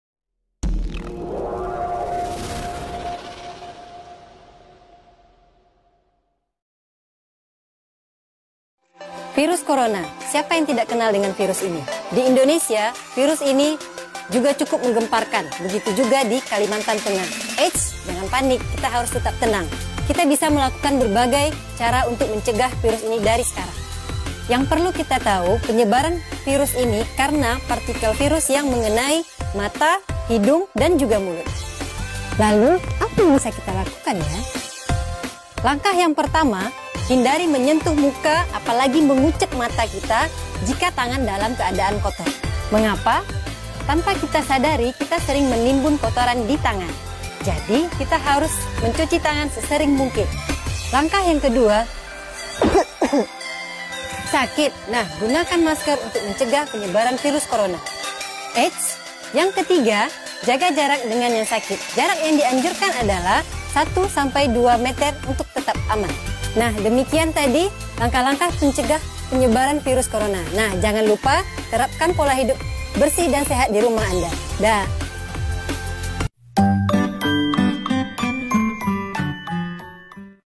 PENCEGAHAN PENYEBARAN VIRUS COVID 19 | DUTA PHBS 2020.
Asalamualaikum wr.wb

Hai teman-teman, kali ini kita uploade video PENCEGAHAN PENYEBARAN VIRUS COVID 19 | DUTA PHBS 2020. Semoga video ini dapat menambah wawasan baik untuk siswa/i SMP Bumitama Sendiri maupun teman-teman dan semoga video ini dapat menjadi tauladan teman-teman  dalam menjaga Kesehatan selama masa pendampingan belajar di rumah.

Akhir kata

Wasalamualaikum wr.wb

"Yayasan bumitama tumbuhkan minat kembangkan bakat berkarakter setulus Hati"

Model : Ana Shonya,S.Pd